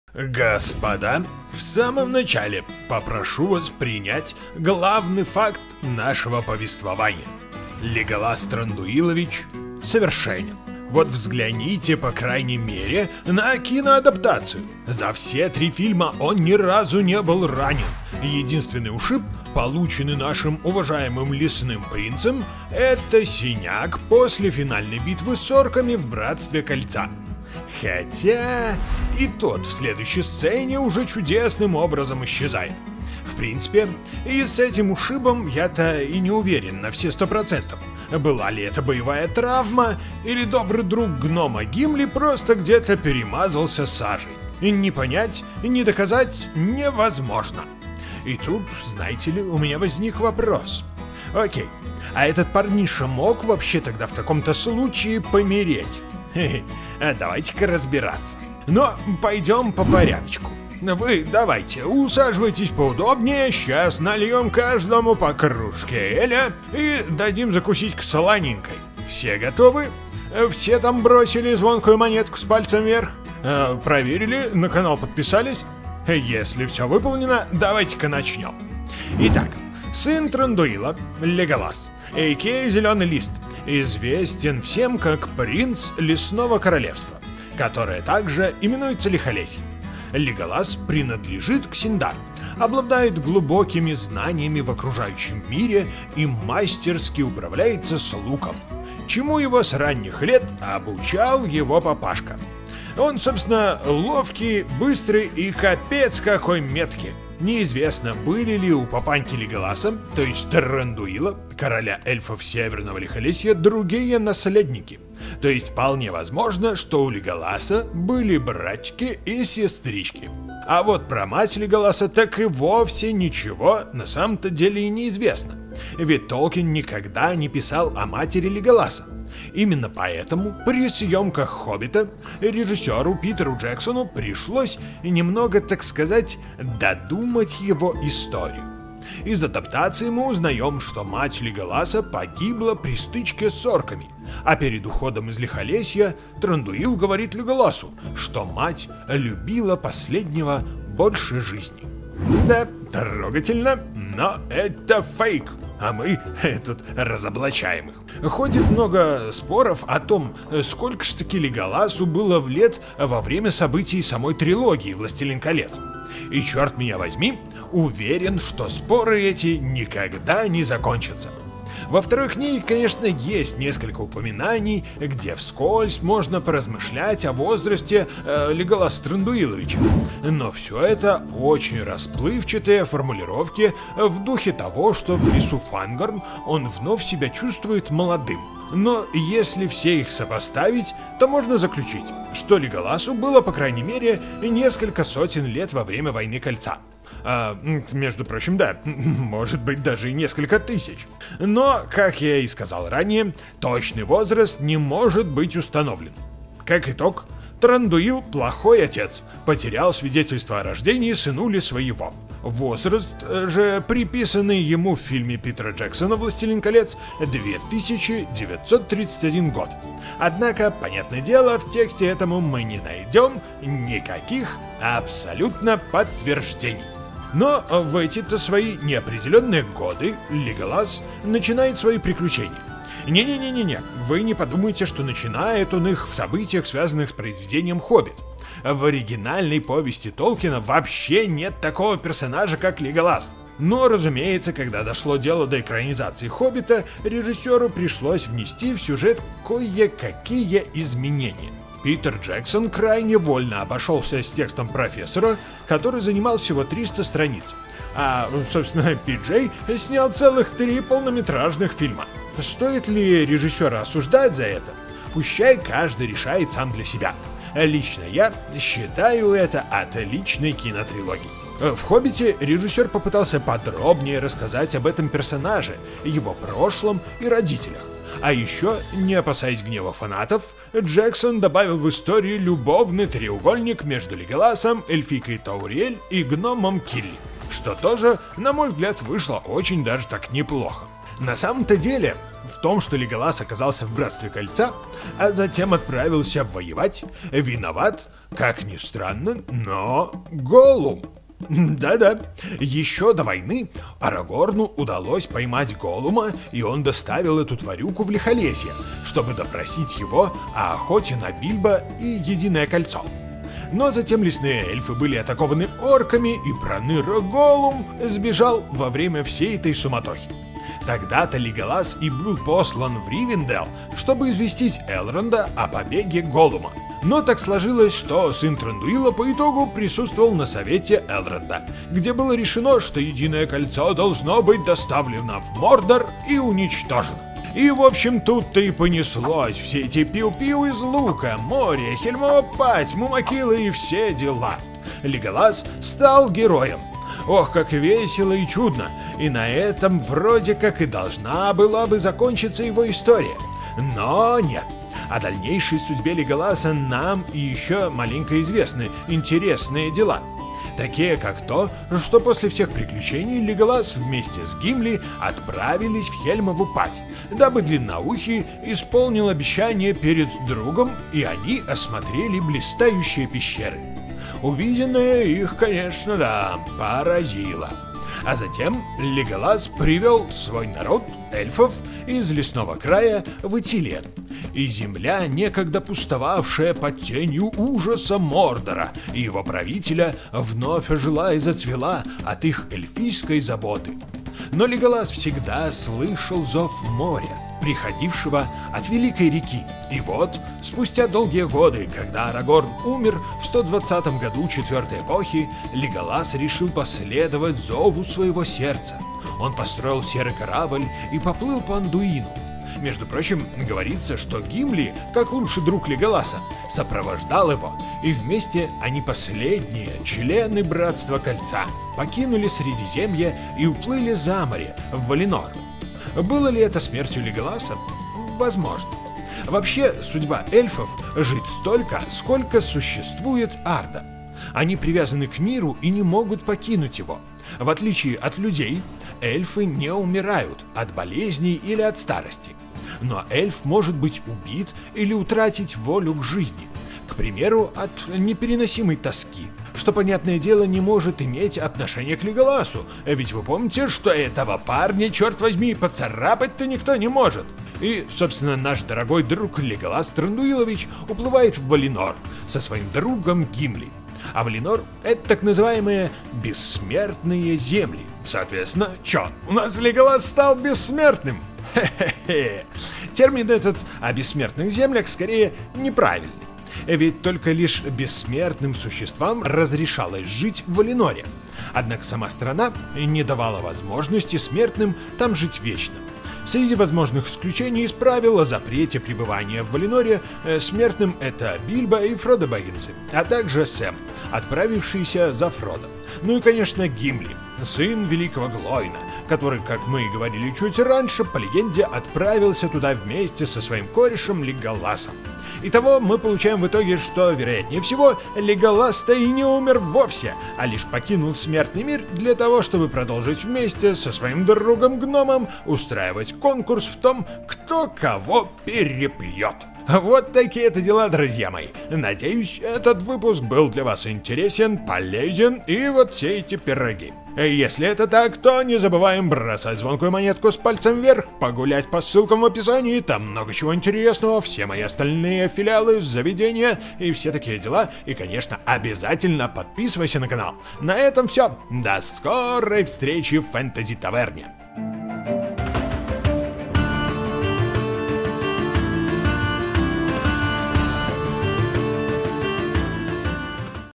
Как умер Леголас? Что стало с эльфом Леголасом после Властелин Колец. Сколько лет было Леголасу?
Как умер Леголас? Что стало с эльфом Леголасом после Властелин Колец? Сколько лет было Леголасу? На эти и многие другие вопросы по вселенной Средиземья мы ответим в новом выпуске по вселенной Толкина в стенах Fantasy Tavern
00:00 ►Бессмертие Леголаса
01:20 ►История Леголаса
02:53 ►Сколько лет Леголасу
04:00 ►Леголас в книге Хоббит
05:00 ►Леголас и Голлум
05:50 ►Как умер Леголас
💎Ролевая игра "Квест".
🏴Сотрудничество, реклама:
Весь материал использованный в данном ролике взят из свободного доступа в сети интернет. Либо является авторским контентом.